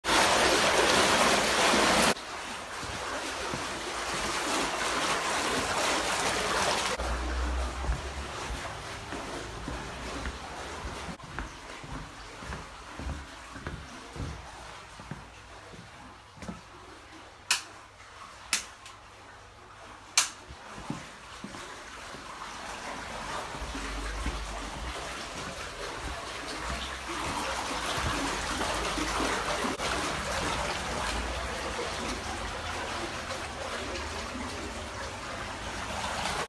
* ANTI - FLOOD. Door barriers, & Anti - flood defence system. VIDEO 2023 10 09 20 49 13
Flat in Scotland one of many, fitted with anti - flood defence system. 3 door barriers, including out building cellar door. & 5 Anti - flood air bricks. This system will secure your home from flooding.